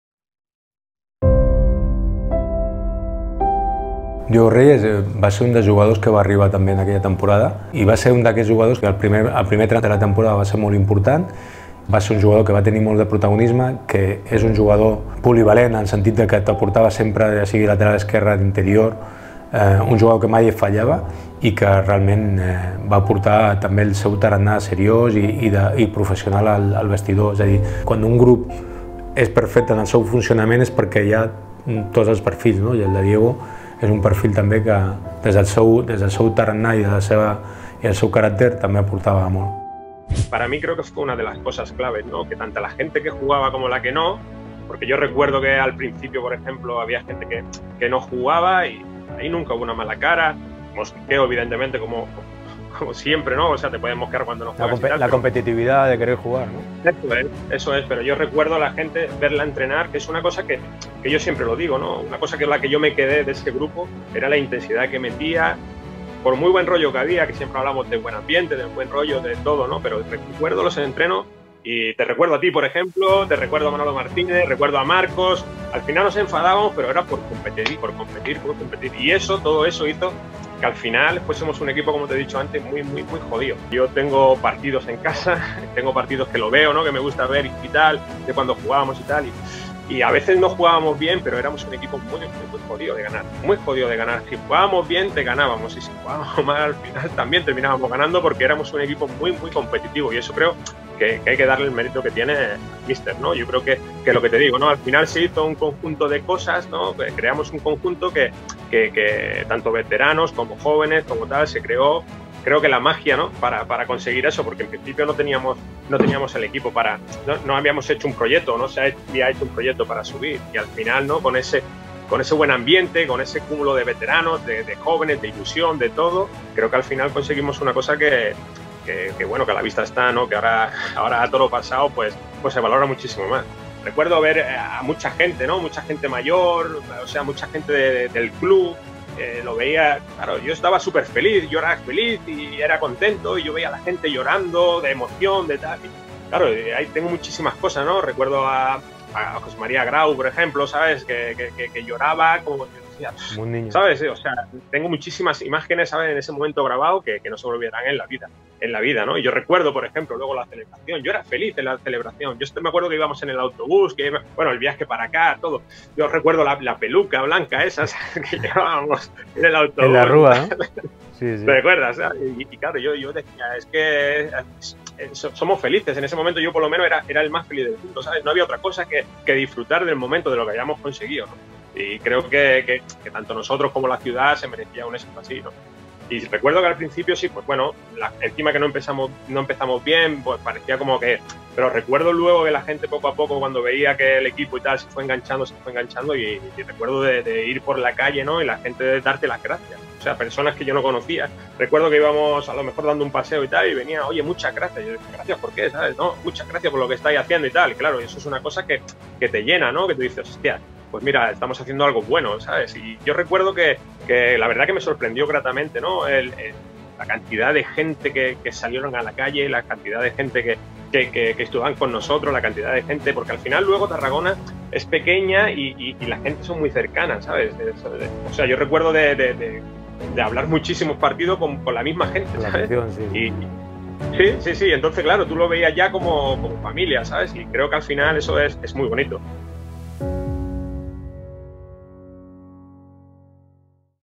Reyes: “La competitivitat al vestidor va ser una de les claus de l’ascens”
Diego Reyes no és un jugador que hagi defensat la samarreta del Nàstic en infinitat de temporades, però l’any que hi va ser es va convertir en el lateral de l’ascens a Primera. El gadità arriba a un vestidor jove, però molt competitiu, un fet que destaca, assegurant que aquesta “va ser una de les claus de l’ascens”.

Reyes recorda especialment la festa de l’ascens a Tarragona, amb els carrers plens de gent, però també recorda especialment amb emoció l’exdelegat del club grana, Josep Maria Grau, a la gespa de Chapín.

Fa quinze anys el Nàstic guanyava a domicili a l’Hércules, per 0-1, amb un gol d’Ismael Irurzun al minut 68. Va ser l’inici d’una marxa triomfal per la categoria, perdent només tres partits més fins acaba la temporada. La màquina del retorn a Primera del Nàstic havia posat la directa.

Diego Reyes Muñoz (Chiclana de la Frontera, 21/07/1979) va estar només una temporada al Nàstic, i va pujar a Primera amb el conjunt grana. Va arribar procedent del Ceuta i posteriorment va marxar al Córdoba, amb qui pujaria a Segona Divisió. També ha jugat amb el Castelló, el Cádiz i el Salamanca, entre altres equips.